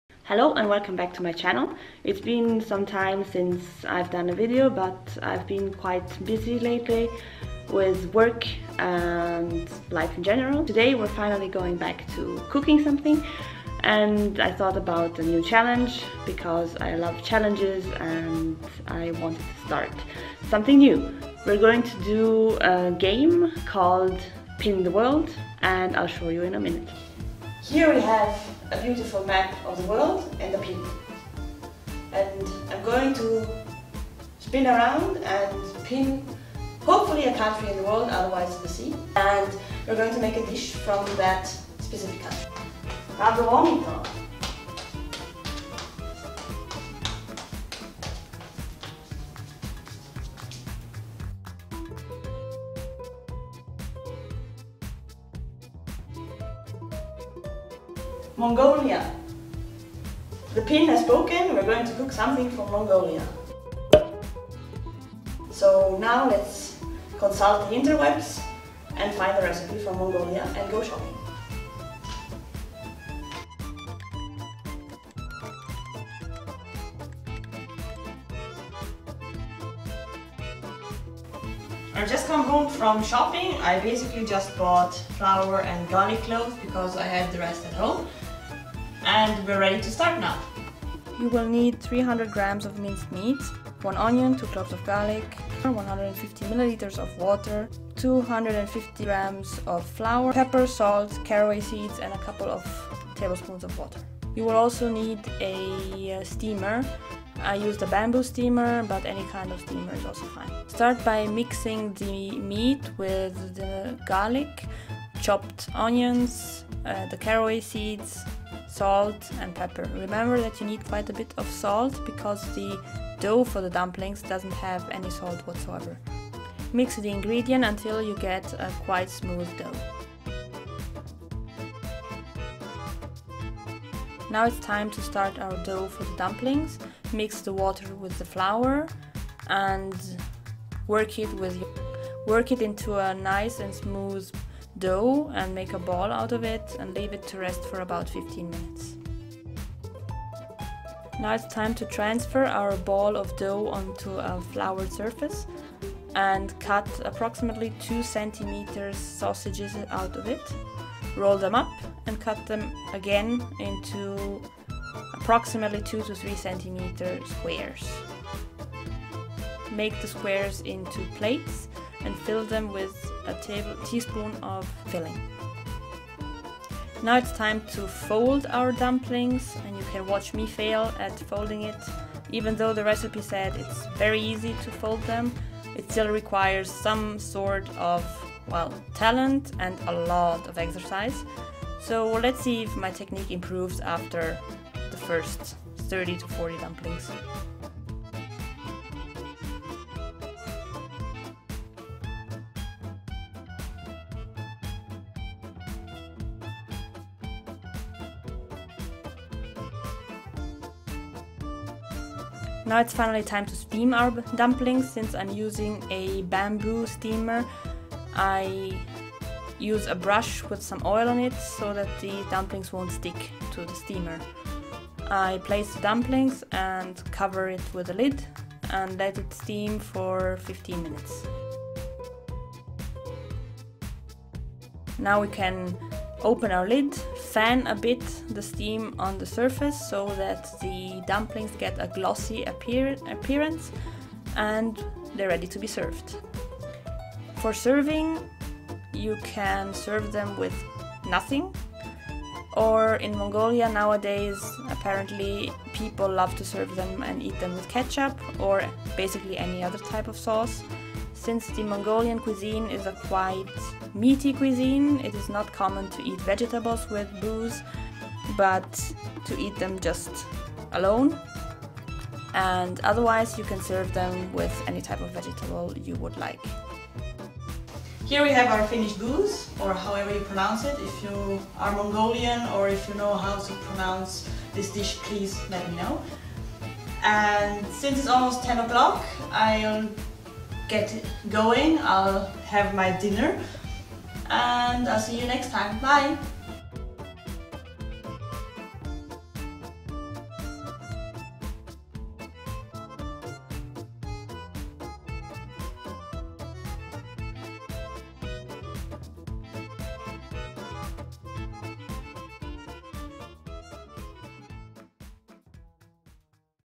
How to make Buuz - aka Mongolian dumplings [Pin the World]
Welcome to "Pin the World"!
Today we're going to make Mongolian Buuz (or trying to...)

Ingredients:

For the dough:

250g flour
150ml water

For the filling:

300g minced mutton (lamb or beef are also OK)
1 onion
2 cloves of garlic
salt
pepper
caraway seeds
3-5 tbsp water